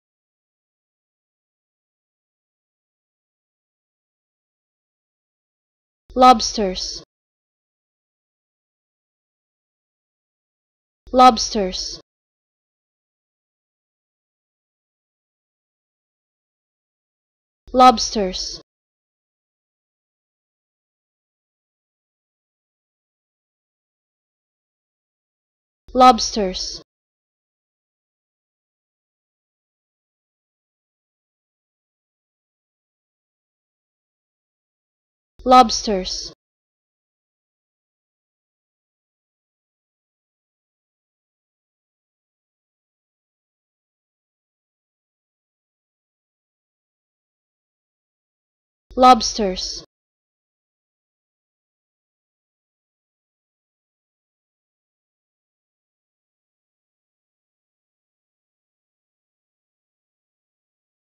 How To Say Lobsters
Learn how to say Lobsters with EmmaSaying free pronunciation tutorials.